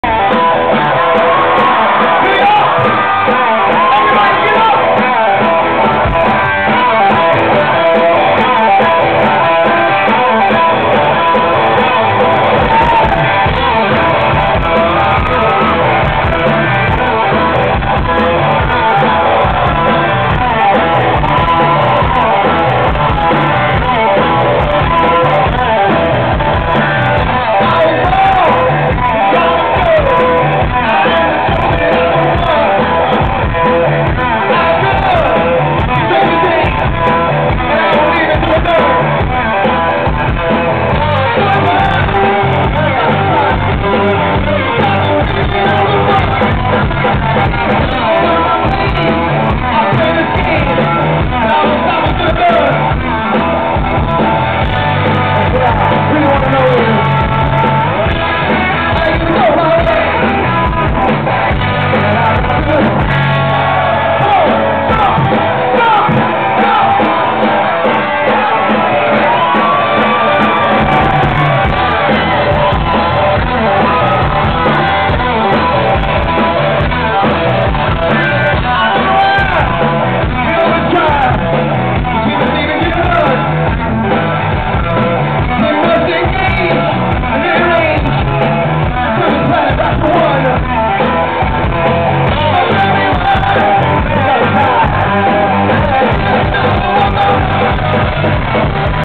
Lenny Kravitz - The Fillmore at Irving Plaza, 10/18/09
Great Show! Lenny Kravitz performs at The Fillmore at Irving Plaza - New York City.  I don't know the name of this song is... if you do, please leave it in the comments so I can update the title - Thanks.